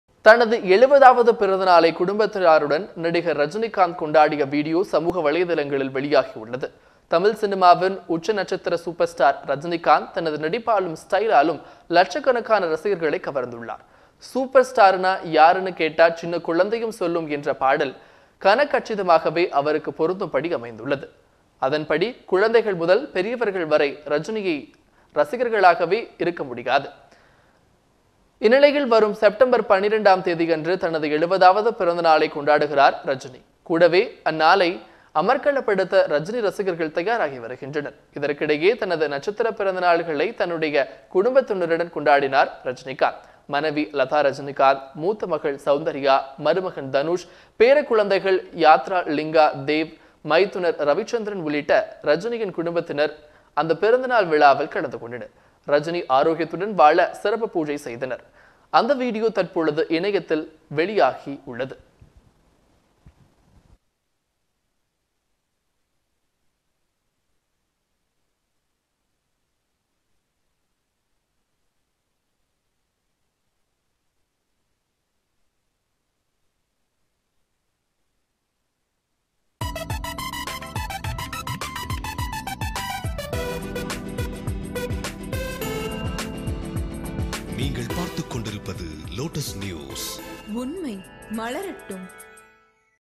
70வது பிறந்தநாளை குடும்பத்தினருடன் கொண்டாடிய நடிகர் ரஜினிகாந்த்
70வது பிறந்தநாளை குடும்பத்தினருடன் கொண்டாடிய நடிகர் ரஜினிகாந்த்.